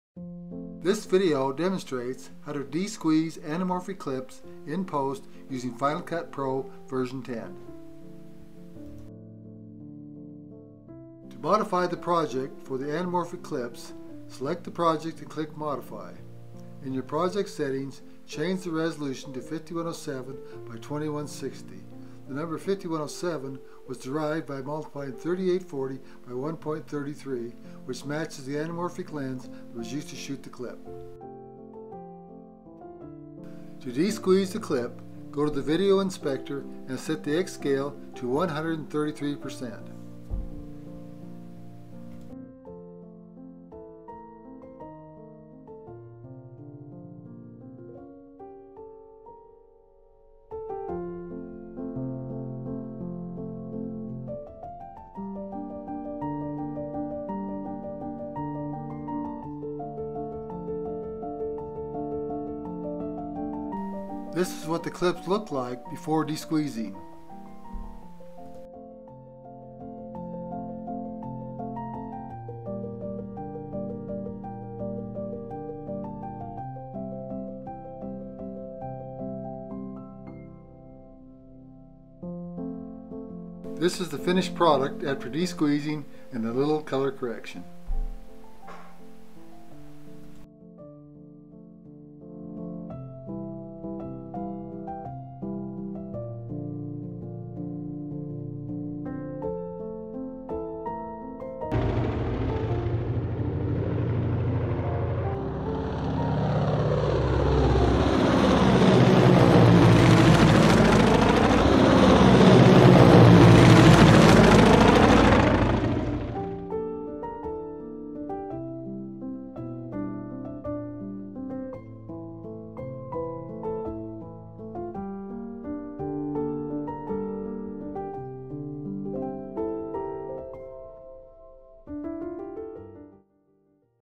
Anamorphic clip de-squeeze using Final Cut Pro x.
iPhone Anamorphic Conversion 4K 10 bit, HLG, Dolby Vision 8 4